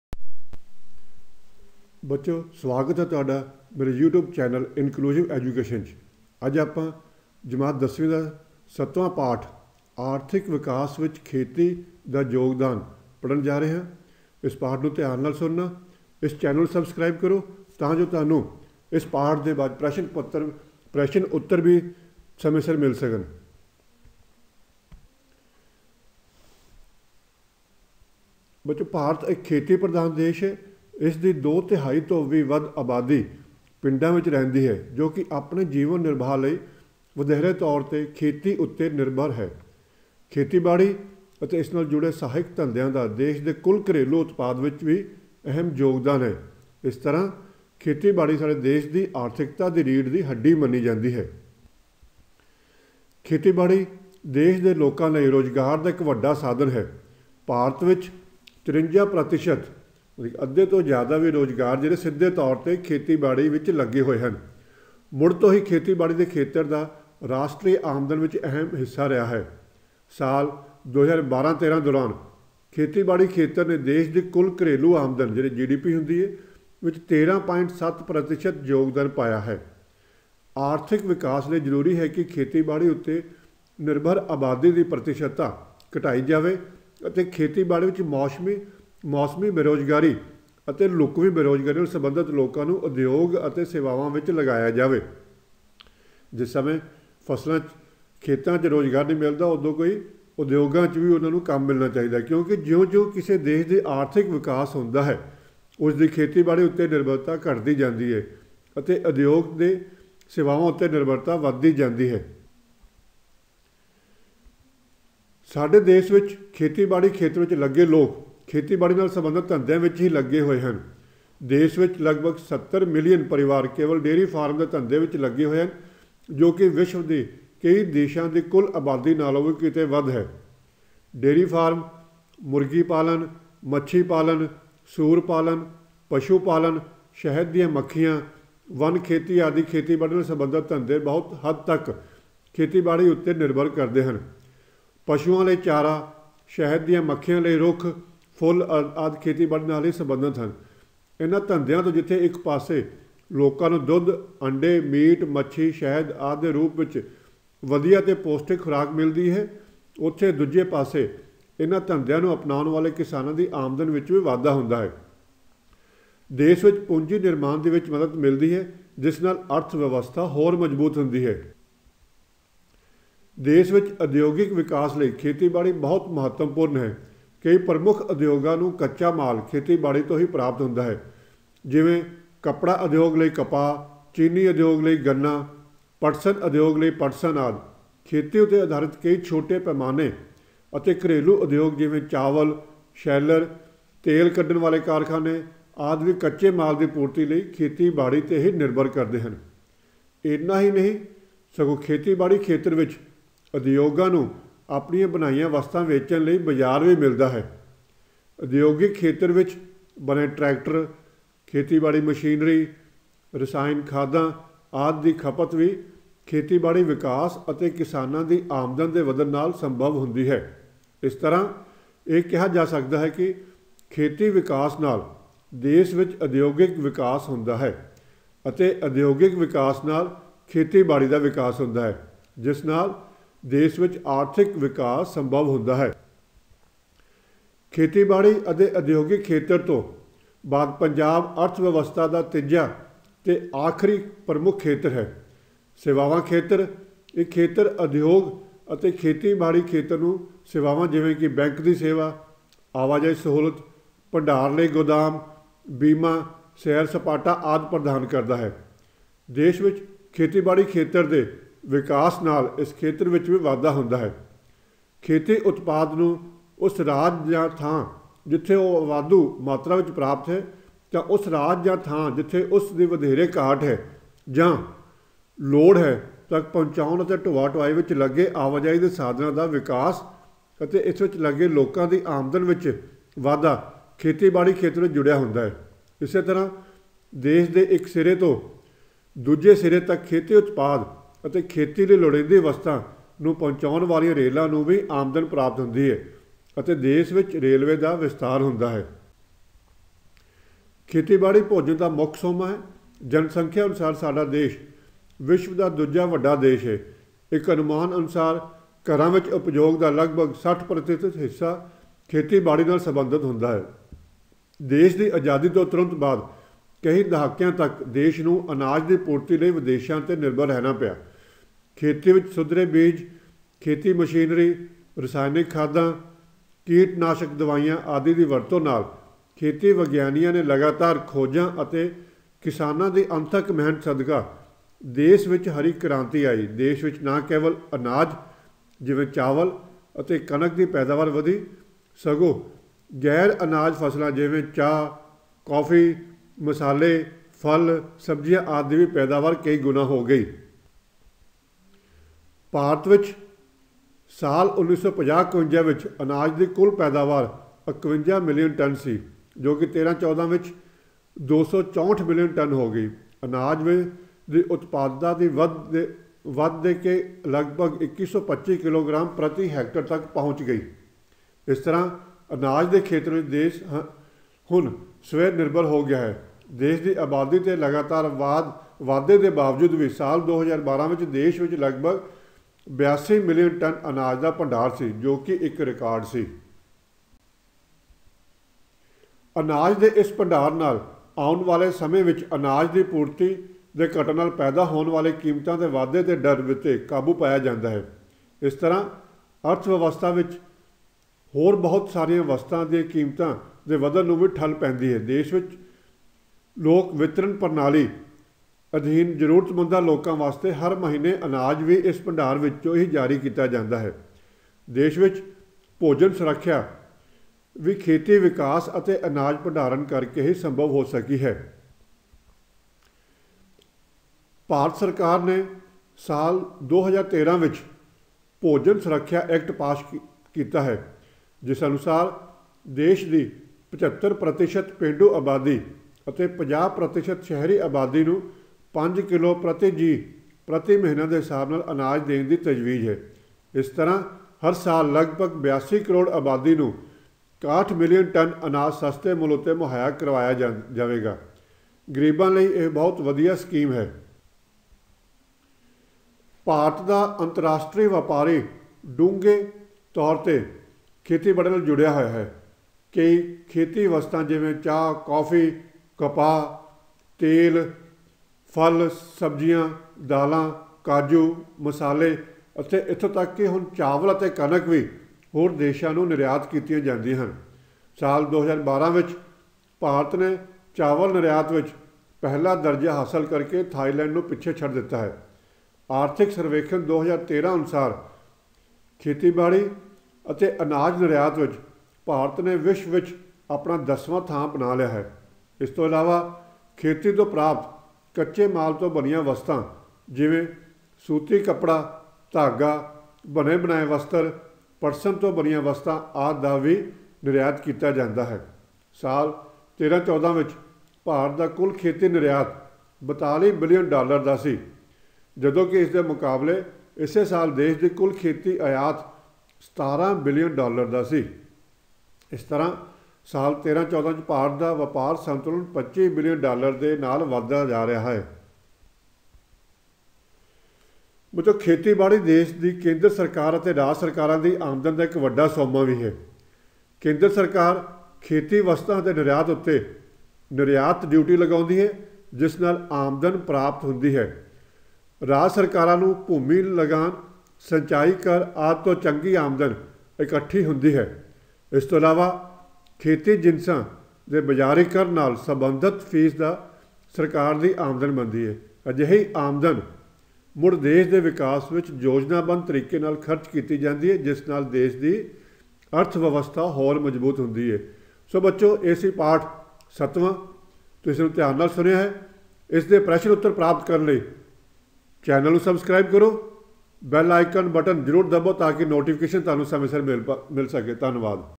Role of Agri in Economic Development ਆਰਥਿਕ ਵਿਕਾਸ ਵਿੱਚ ਖੇਤੀ ਦਾ ਯੋਗਦਾਨ Class tenth chapter seventh
Role of Agriculture in Economic_Development ਆਰਥਿਕ ਵਿਕਾਸ ਵਿੱਚ ਖੇਤੀ ਦਾ ਯੋਗਦਾਨ Class tenth chapter seventh